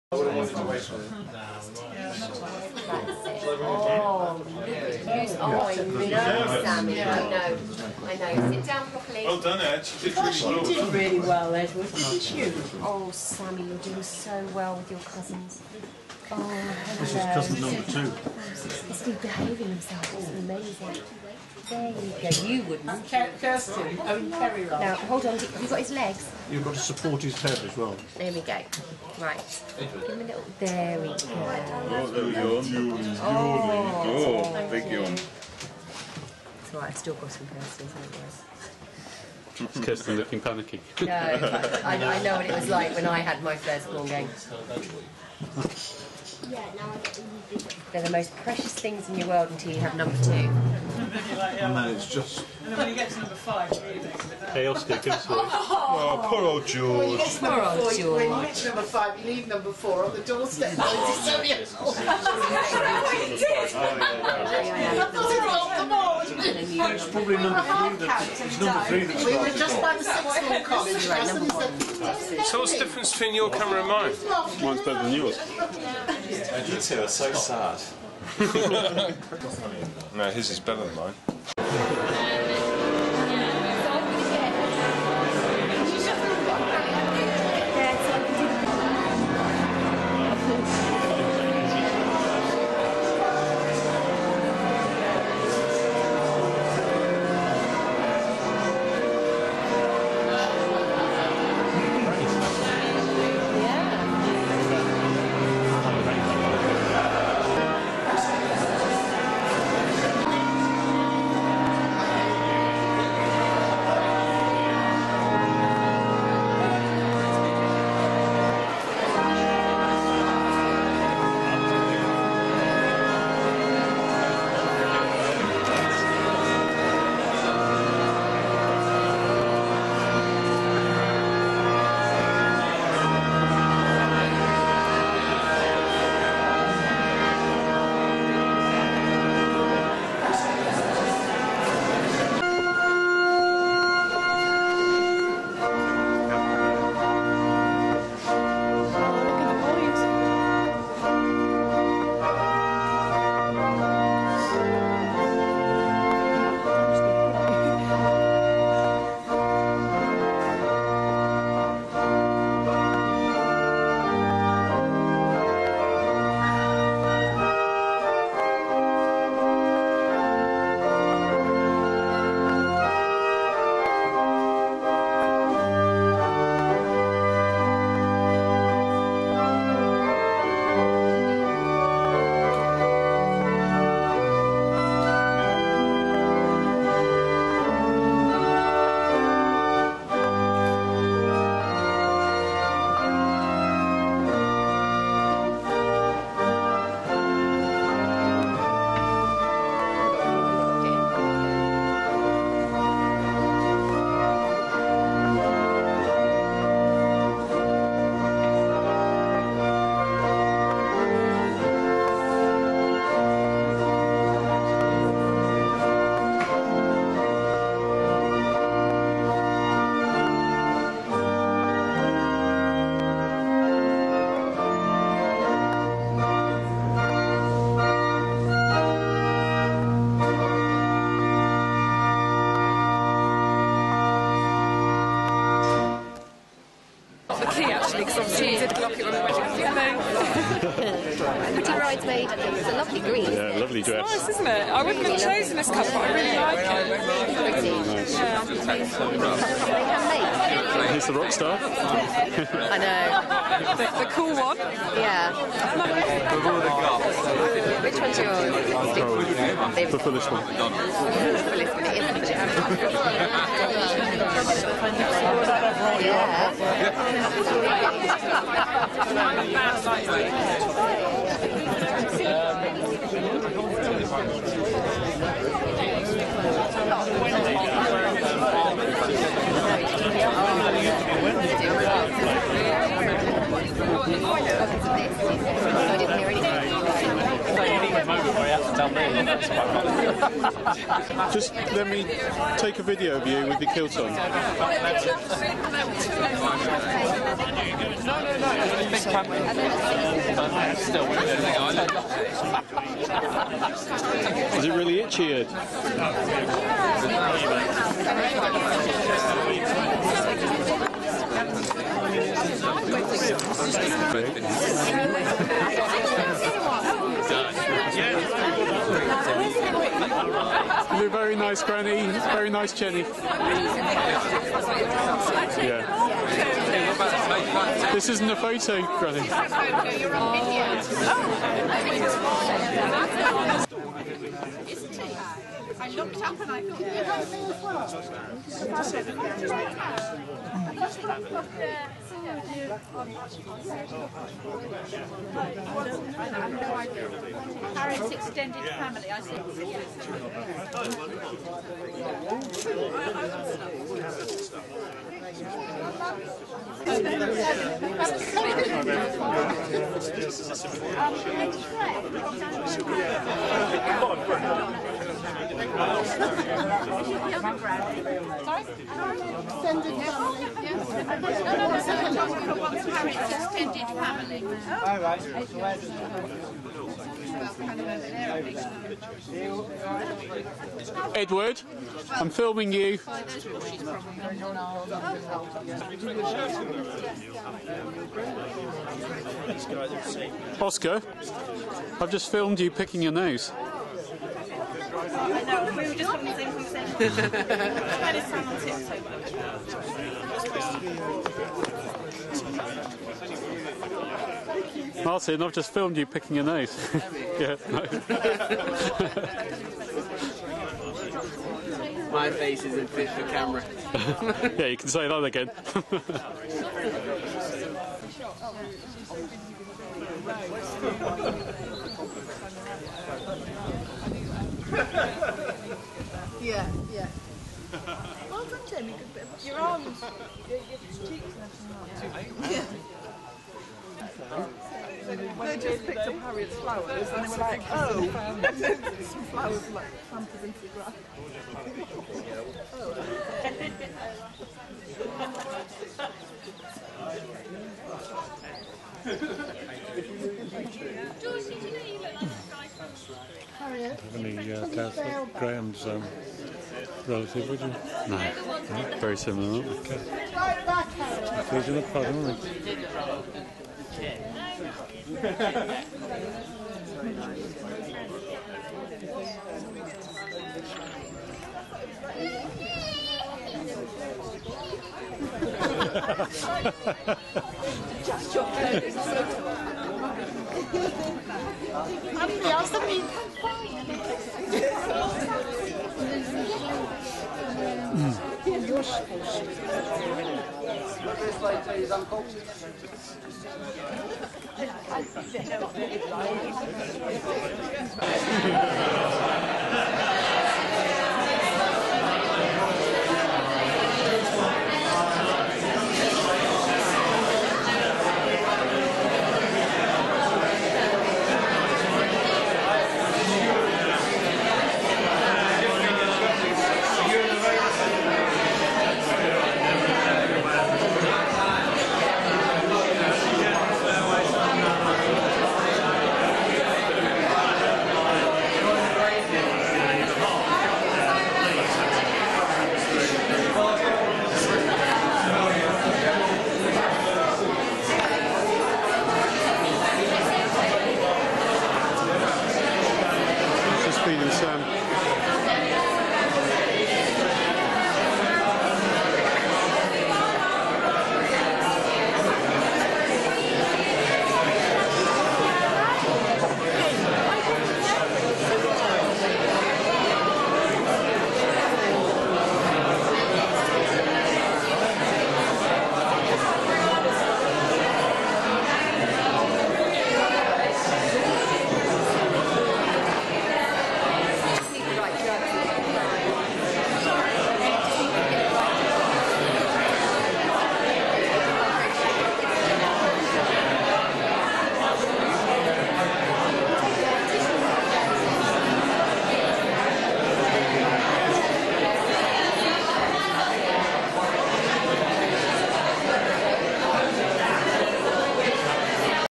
The marriage of Harriet Elizabeth and Mr. Graham Beveridge Pt1
Dinner night before wedding, Harriet walking down aisle, family photo's and wedding breakfast.